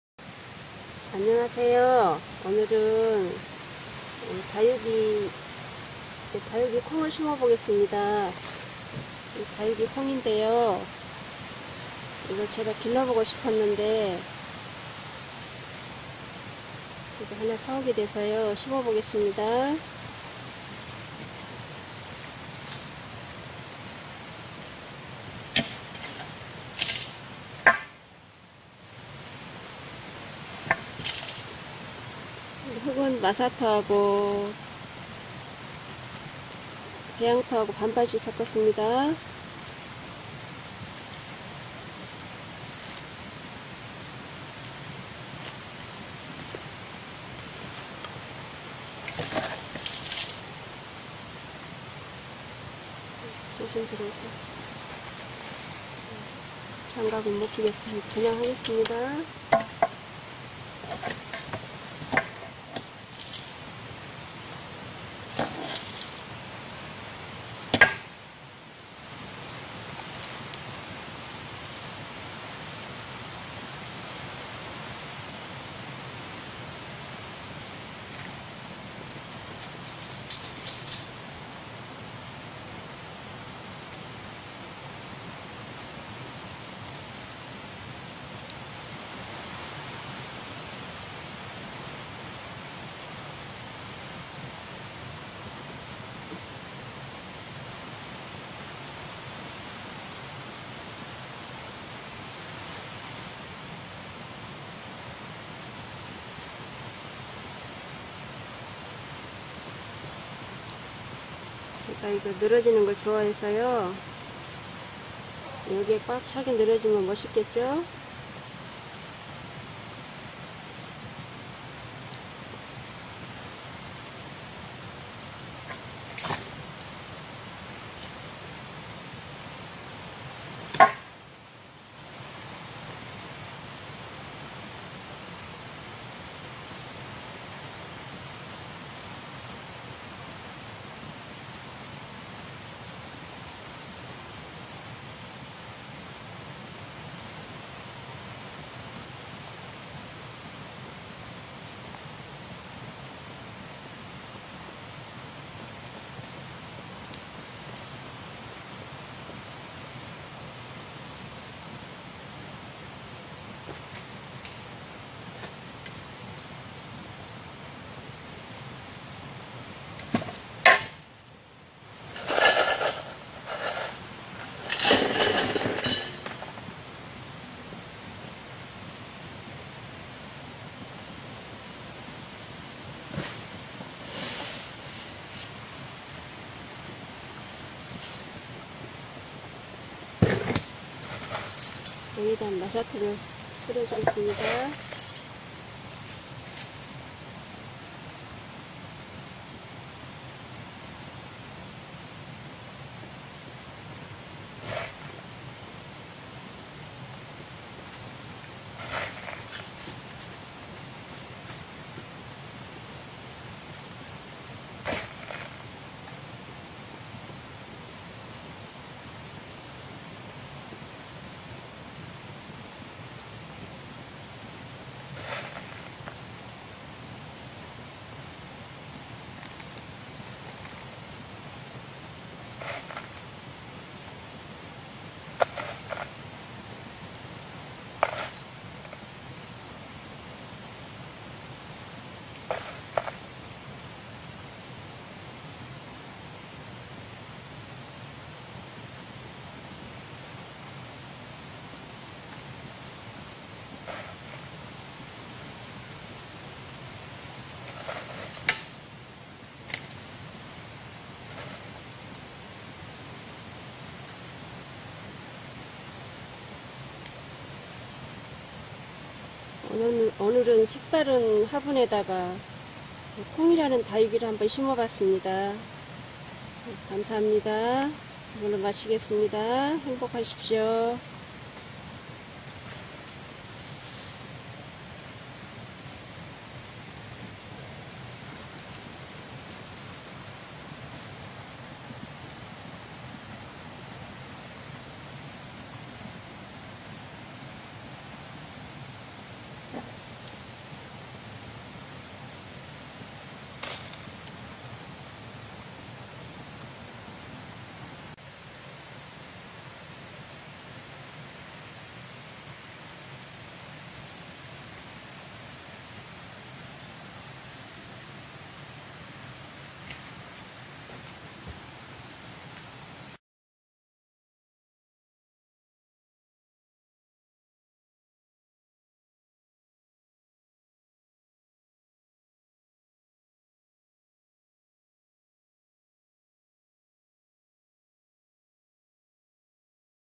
친절한광자씨 코코넛화분에 콩란다육을예쁘게 심어봤습니다
영상중에 콩다육이라고 설명드렸는데 이름이 콩란이라고 정정 합니다